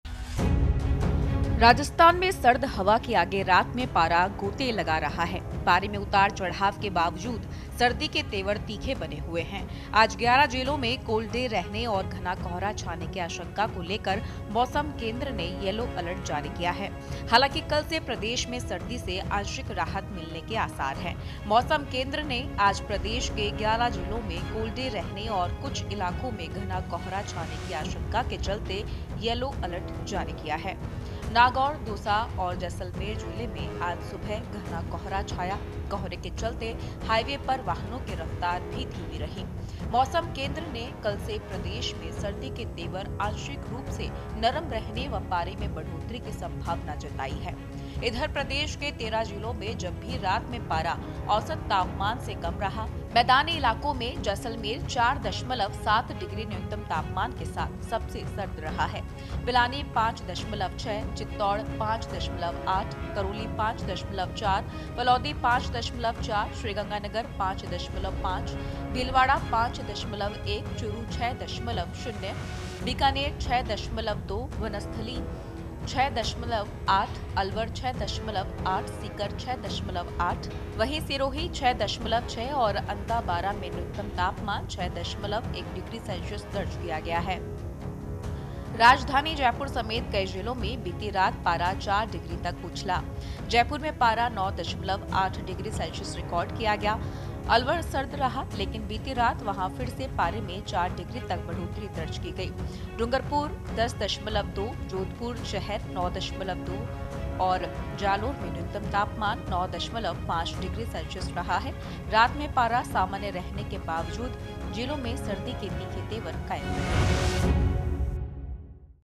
Weather Update Today: IMD का बड़ा अलर्ट, 11 जिलों में कोल्ड डे का Yellow Alert | Weather Latest News
Weather Update Today : IMD का बड़ा अलर्ट, 11 जिलों में कोल्ड डे का Yellow Alert, उत्तरी भारत में कड़ाके की सर्दी, कोहरा और शीतलहर का डबल अटैक | Weather Latest News | Today Mausam Update | Breaking News

समूचे भारत में कड़ाके की सर्दी का दौर बना हुआ है। राजस्थान में सर्द हवा के आगे रात में पारा गोते लगा रहा है। मौसम में उतार-चढ़ाव के बावजूद सर्दी के तेवर तीखे बने हुए हैं। आज 11 जिलों में 'कोल्ड डे' और घने कोहरे का यलो अलर्ट जारी किया गया है।